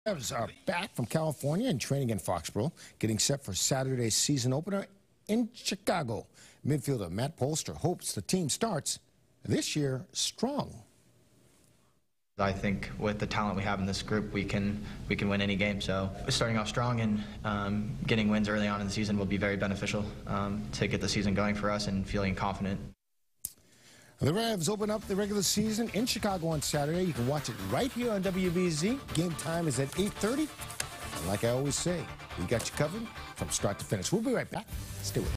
Revolution Training In Foxboro For Saturday's Season Opener
The New England Revolution are preparing for their season opener against the Chicago Fire.